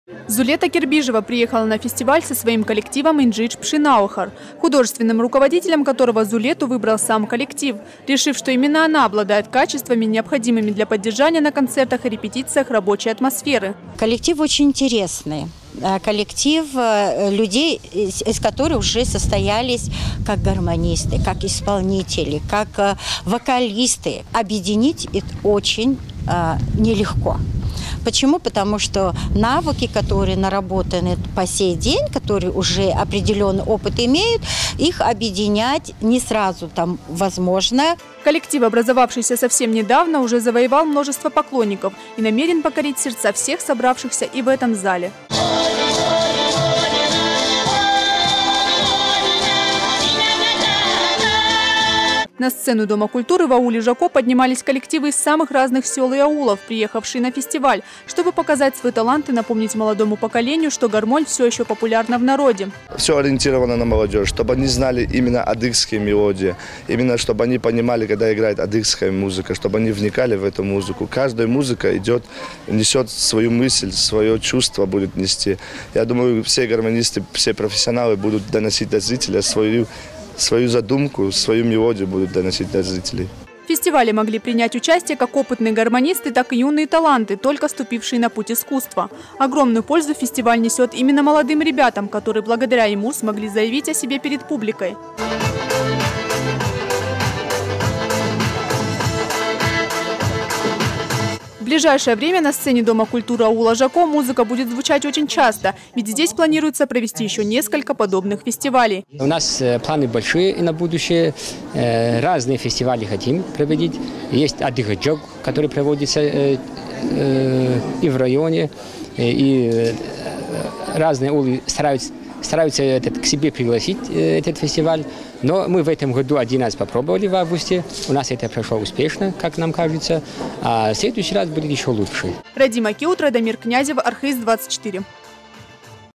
Дом культуры аула Жако распахнул двери перед участниками фестиваля «Адыгская гармоника»
Дом культуры аула Жако распахнул двери перед участниками фестиваля гармонистов Хабезского района «Адыгэ Пшыналъэ», что в переводе с черкесского означает «Адыгская гармоника». В программе фестиваля были заявлены как сольные выступления, так и ансамбли.